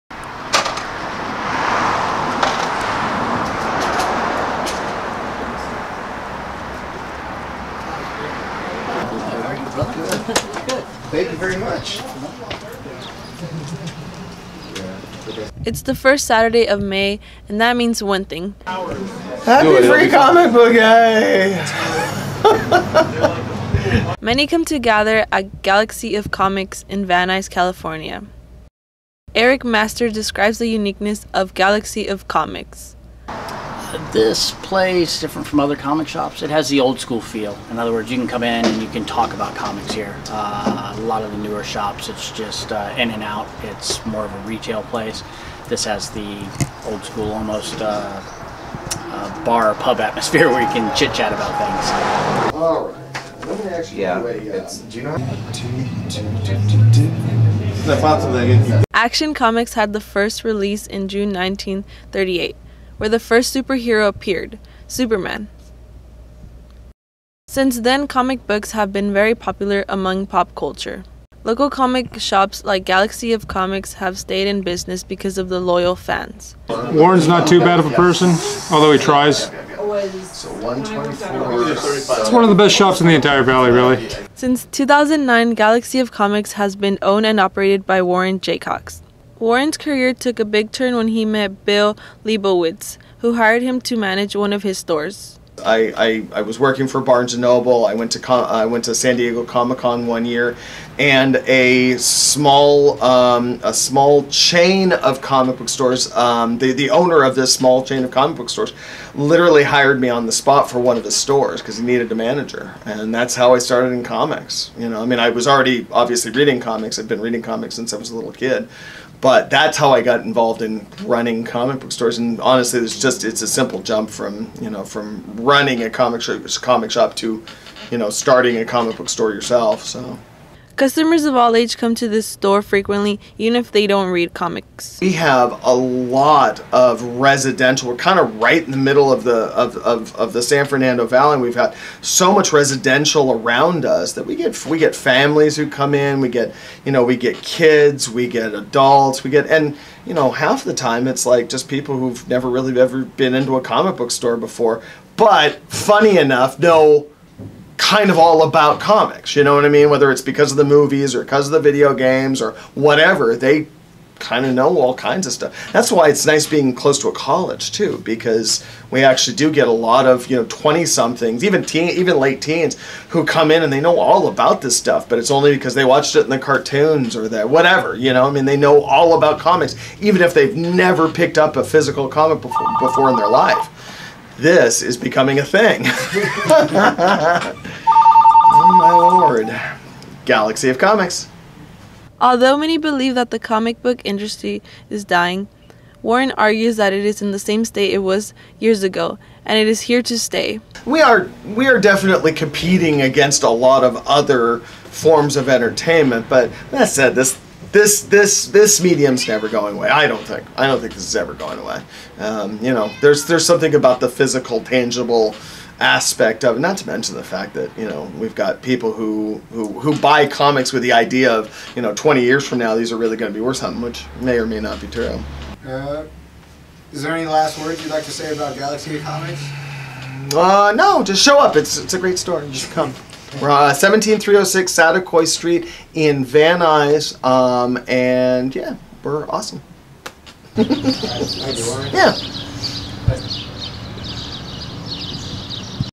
Galaxy of Comics
Documentary on Galaxy of Comics, a small comic shop located in Van Nuys, California.